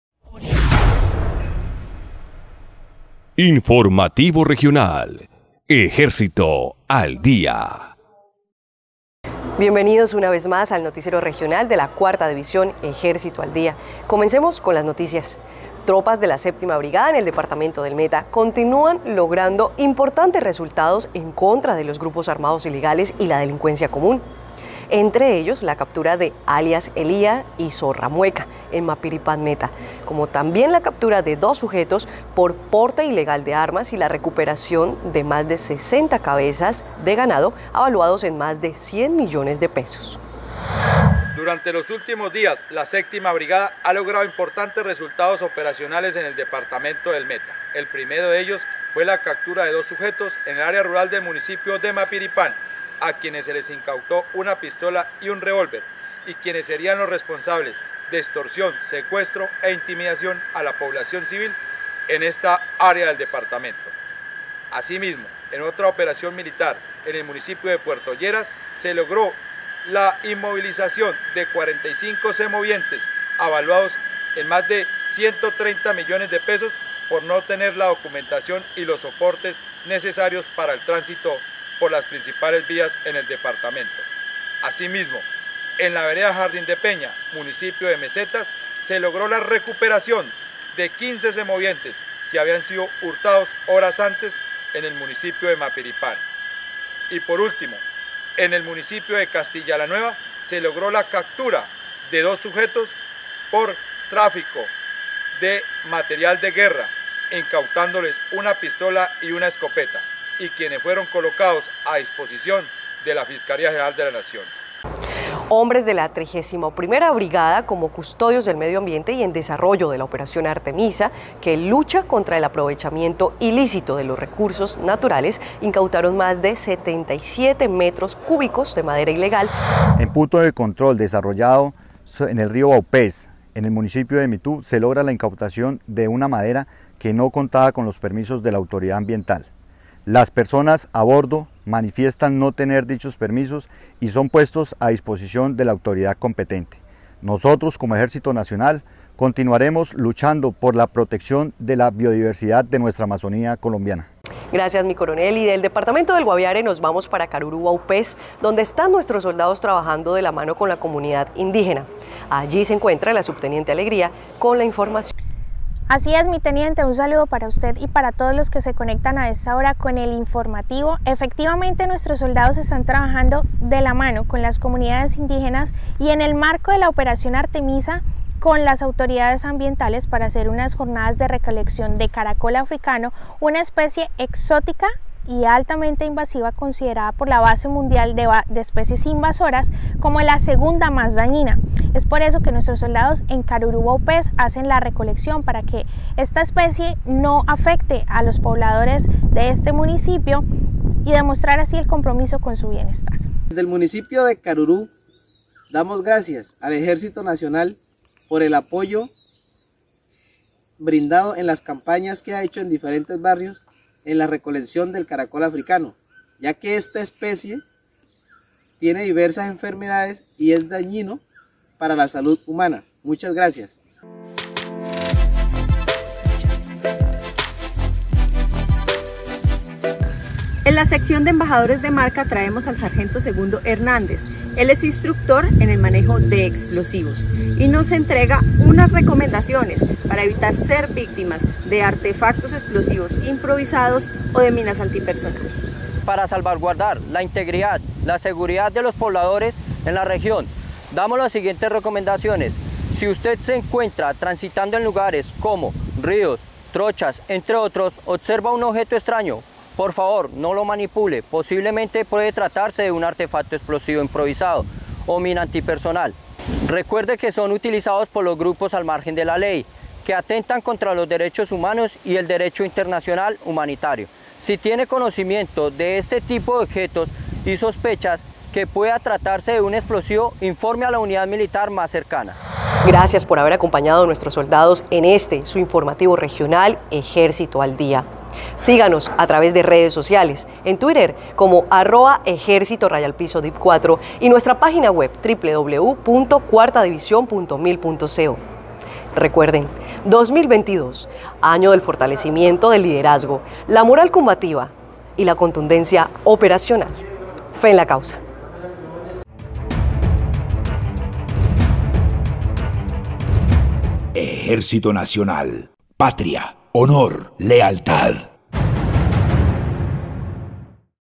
Informativo Regional Ejército al Día
Esta semana en el Informativo Ejército al Día, entérese de los resultados más destacados en el departamento del Meta; en esta región los soldados enfrentan delitos como el narcotráfico y el abigeato.

Así mismo, conozca como las tropas atacan el delito de la deforestación en el Bioma Amazónico del departamento de Vaupés.